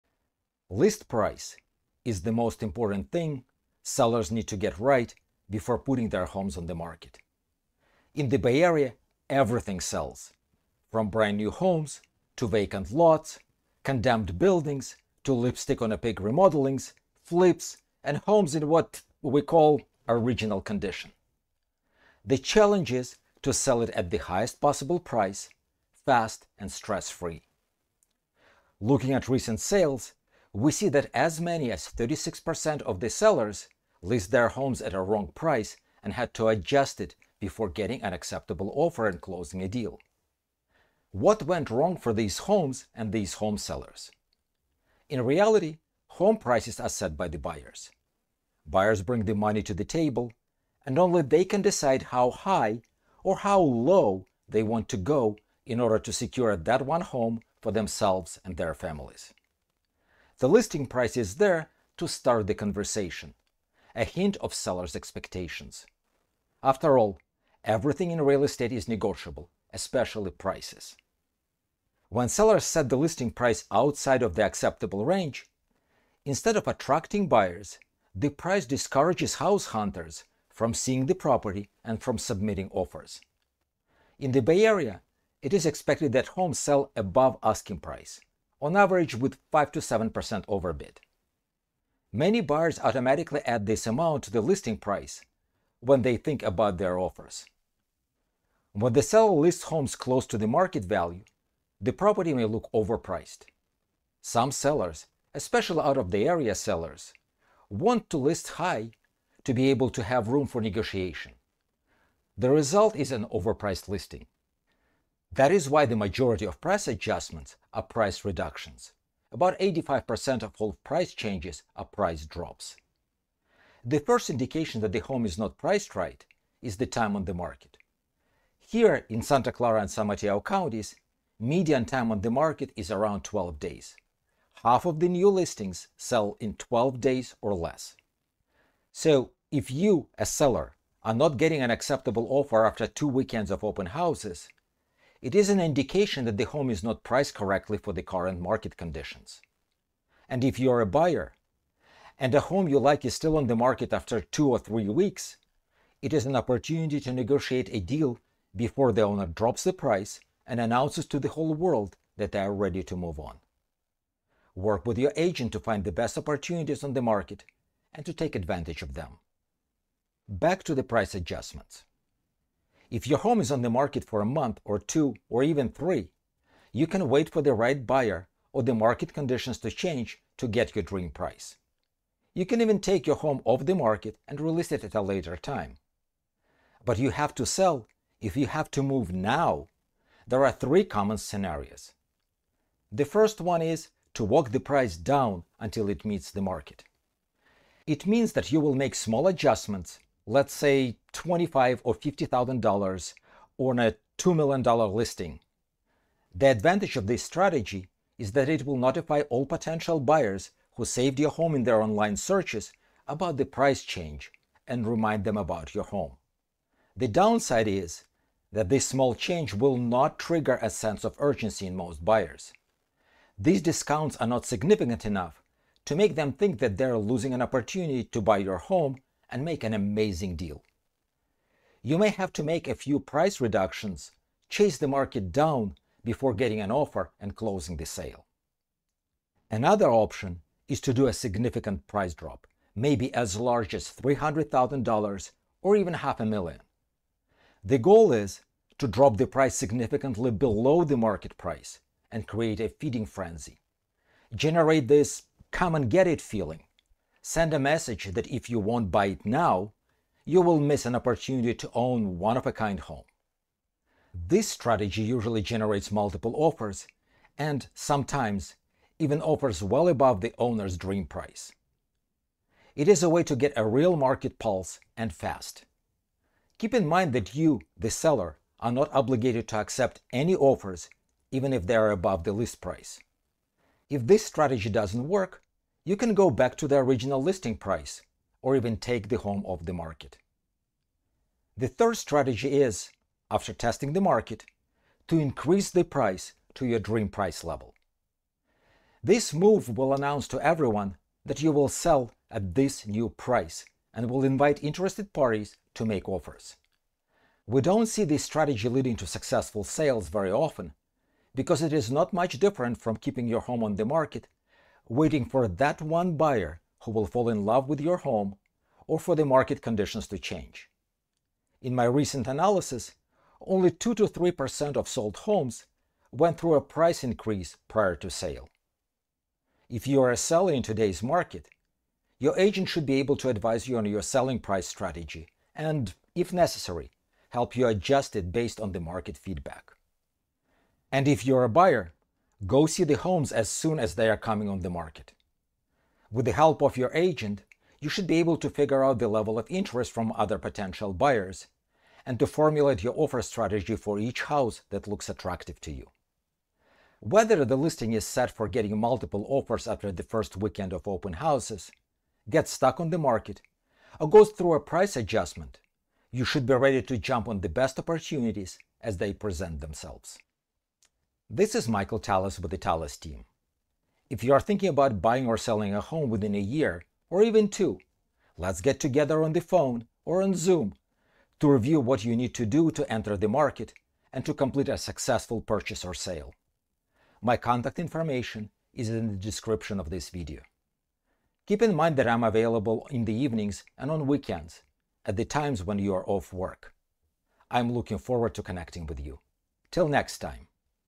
Sell Your Bay Area Home: A Strategic Approach to Pricing for Buyers and Sellers
In this episode, Michael Talis from the Talis Team discusses the critical importance of setting the correct list price when selling homes in the Bay Area. He emphasizes that the list price sets the tone for negotiations and highlights the need for strategic pricing adjustments when homes aren't selling within the typical market timeframe. He outlines various pricing strategies including gradual reductions, significant price drops to incite buyer urgency, and rare price increases. The episode advises sellers and buyers on navigating market conditions effectively and stresses the importance of timing and proper guidance from real estate agents to ensure successful transactions.

00:00 The Importance of Setting the Right List Price
00:45 Understanding Buyer Behavior
01:44 Common Pricing Mistakes
02:03 Indicators of Incorrect Pricing
02:52 Reasons for Price Adjustments
03:13 Walking Price Down
03:59 Get Ahead of the Market
04:51 Get It at This Price Strategy
05:29 Working with Your Agent
06:10 Conclusion and Contact Information